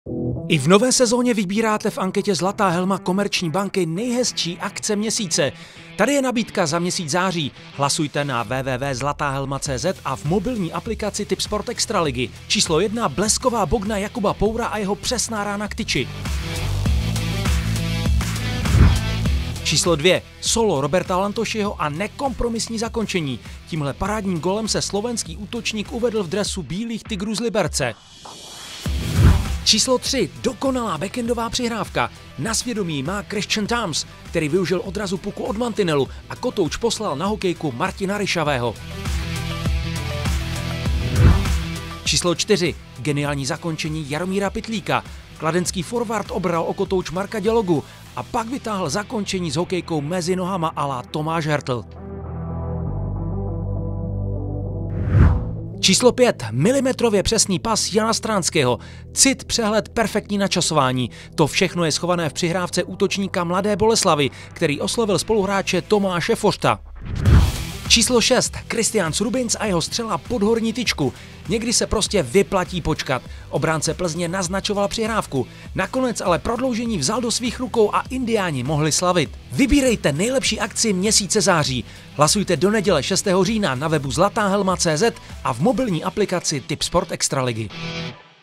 Zlatá helma Komerční banky v září 2024
Tipsport extraliga 2024/2025 nabírá neskutečné obrátky a je tedy čas ohlédnout za prvním měsícem a jeho nejhezčími akcemi. O přízeň fanoušků se bude ucházet 6 TOP akcí, ale jen jednu z nich můžete svým hlasováním poslat do velkého finále, které se uskuteční na konci sezony.